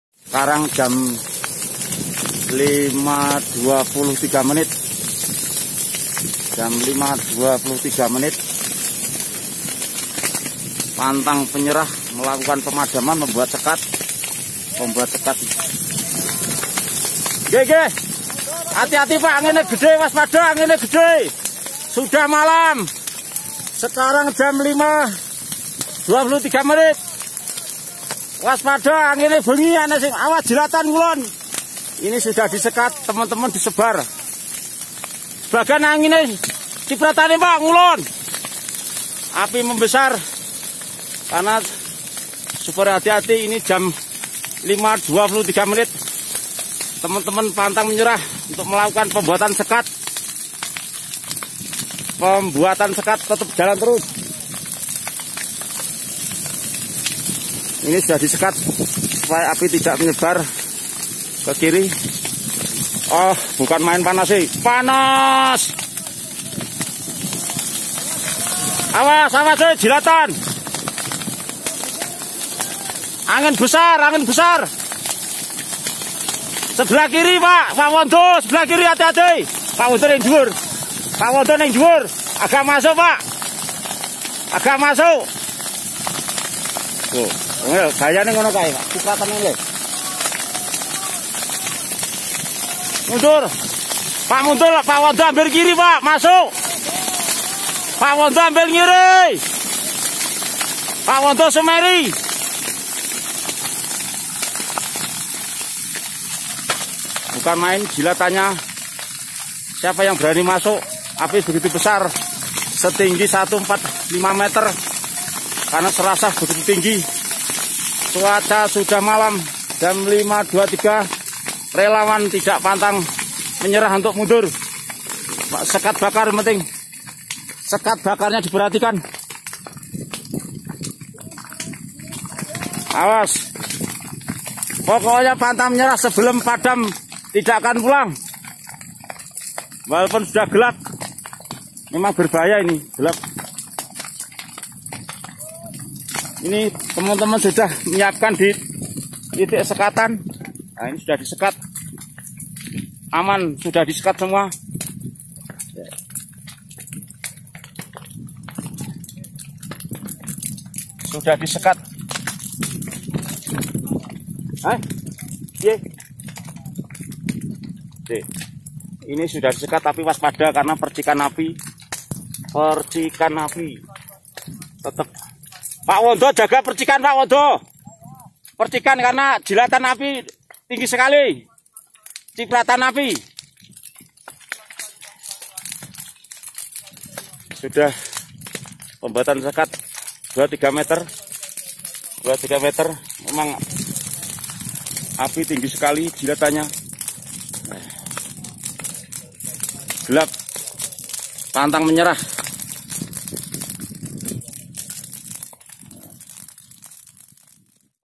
Gambaran Perjuangan Tim Pemadam Kebakaran Gunung Merbabu
Berikut ini video yang saya terima langsung dari pak Sukimin, Polhut Balai TNGMb, Resort Selo, yang diambil pada Senin (16/9/2019). Ini berada pada salah satu titik api di sekitar jalur pendakian Selo. Menggamabarkan perjuangan para tim pemadam dalam usaha memadamkan api yang membajar kawasan gunung Merbabu.

Video ini untuk mendukung artikel atau tulisan saya tentang "Mereka yang Berjibaku Memadamkan Amukan Api di gunung Merbabu" di Kumparan.com